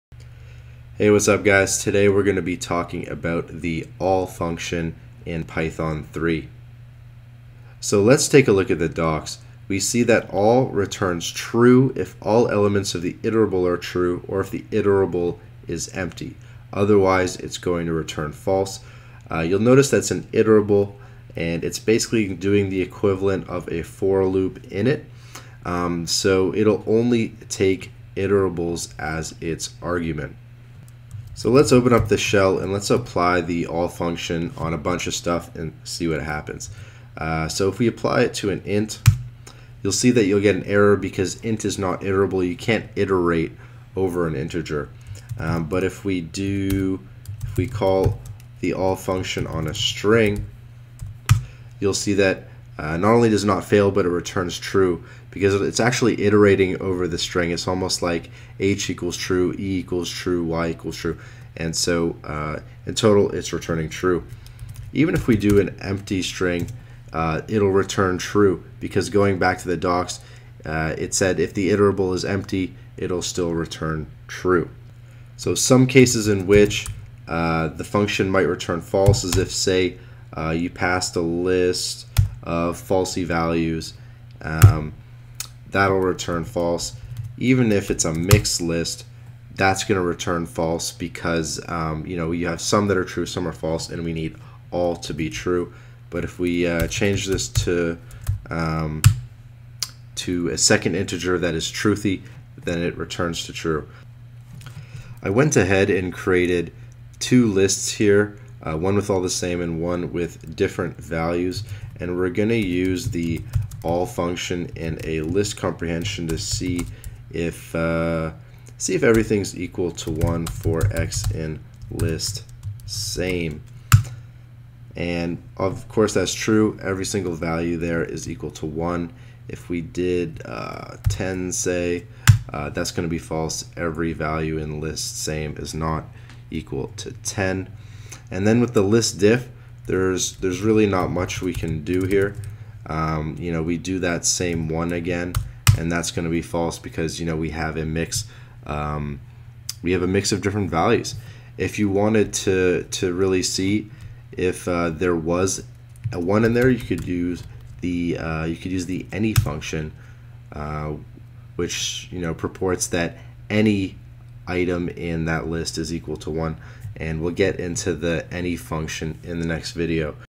Python 3 all() built-in function TUTORIAL
Tutorial on how to use the all() built-in function from the Python 3 Standard Library.

Timestamps:
0:00-0:30 Documentation
0:31-1:43 All() basics
1:44 All() with List Comprehension